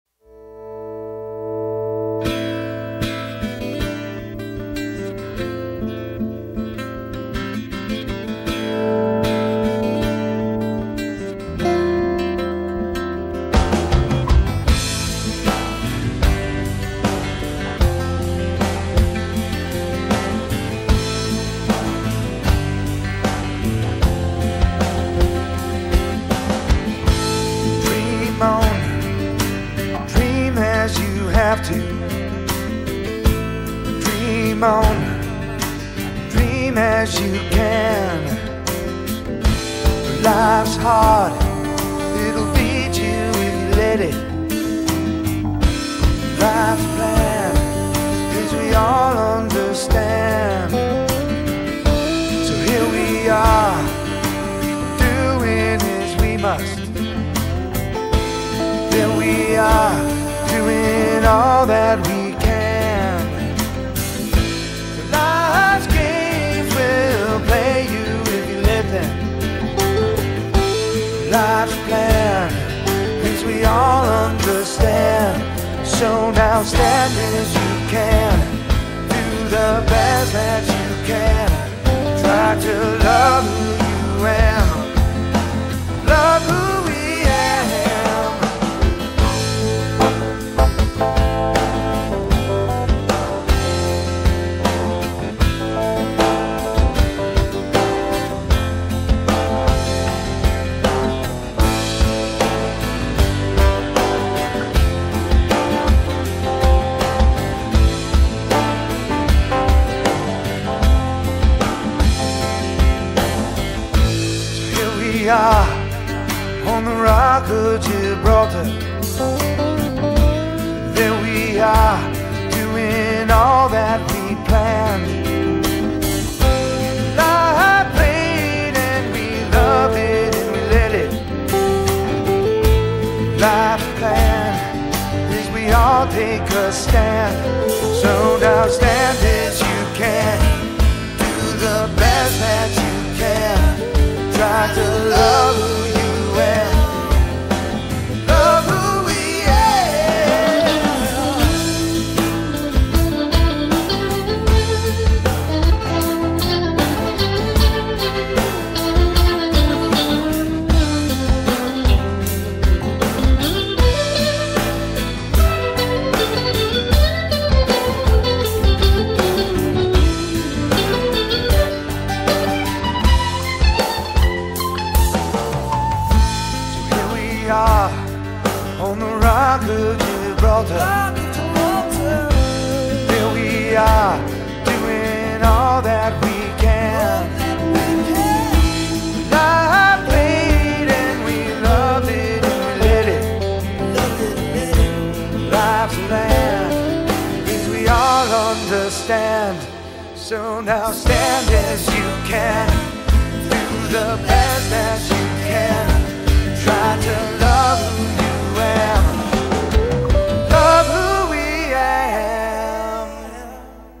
A photo tour of Gibraltar on the tip of Spain
Visit this British Overseas Territory - a unique place on the tip of Spain.